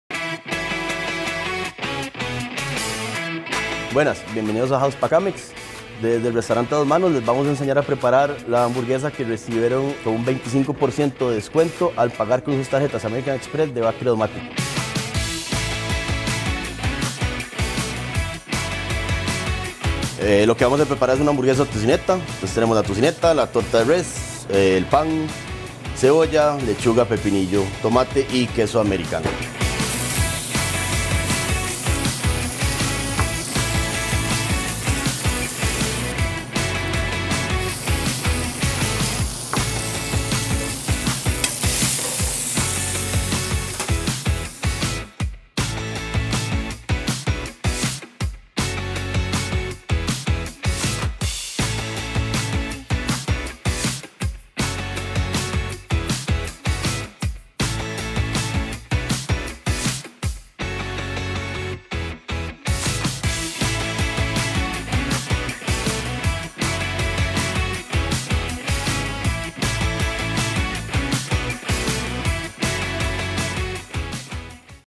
House Pack Amex - A dos manos
American Express® BAC Credomatic y Bavaria te traen House Pack Amex. ¡Listo para prepararlo en casa! Los mejores platillos de tus restaurantes favoritos.
Ingresá a mipromo.com y comprá tu cupón